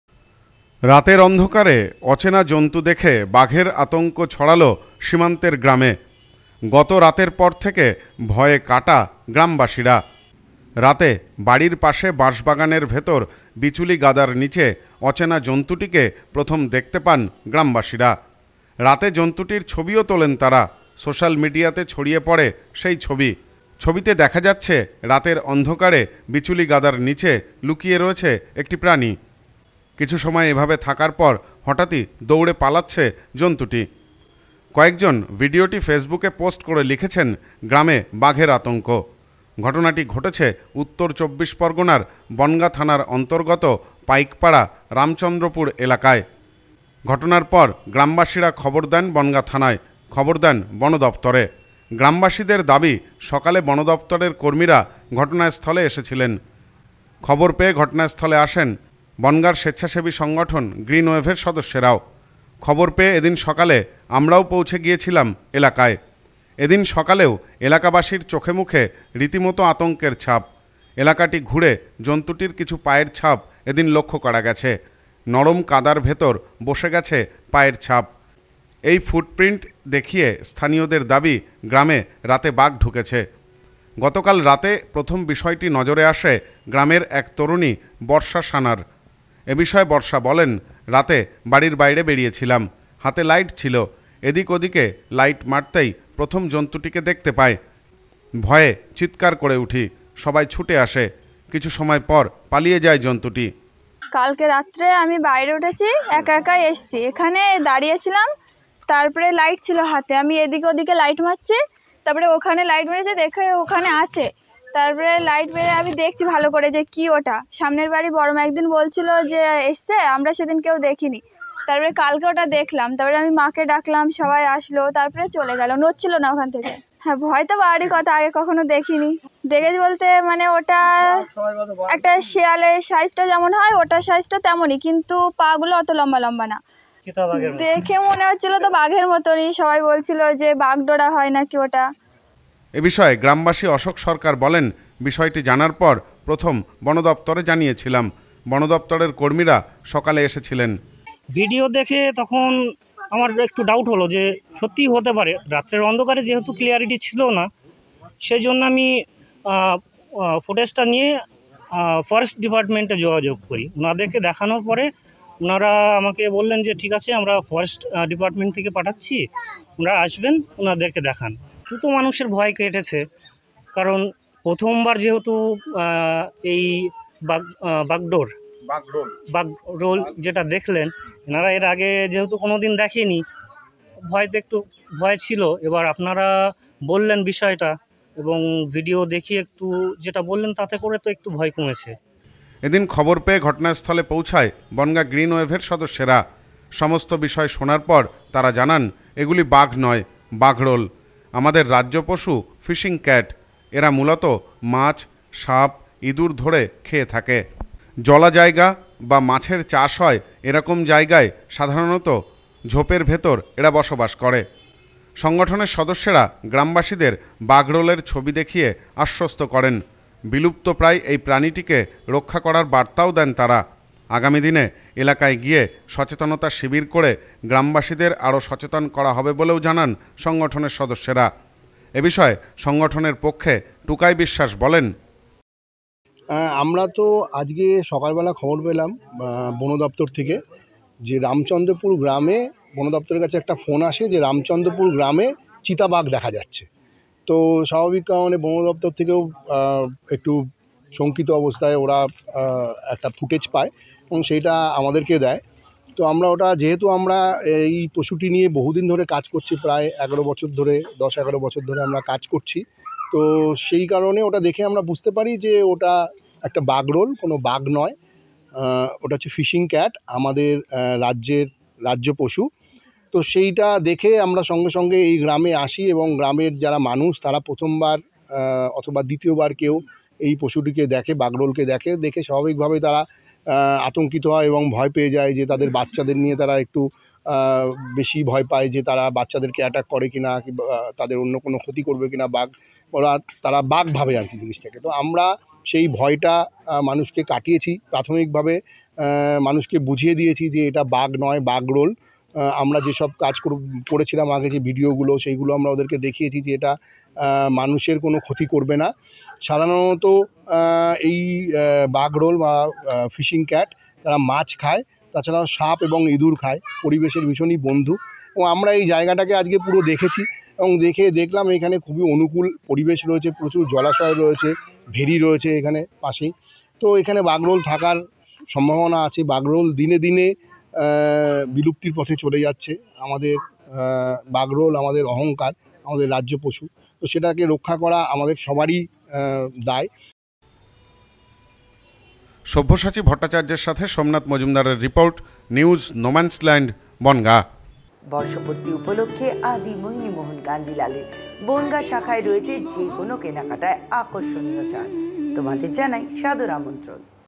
গায়ে ডোরাকাটা, অচেনা জন্তু দেখে বাঘের আতঙ্ক বনগাঁর গ্রামে!
গায়ে ডোরাকাটা, অচেনা জন্তু দেখে বাঘের আতঙ্ক বনগাঁর গ্রামে! জন্তুটিকে চিতাবাঘ বলে দাবি করে ফেসবুকে পোস্ট। বাঘ নয়, বাগরোল গ্রামবাসীদের সচেতন করল স্বেচ্ছাসেবী সংগঠন।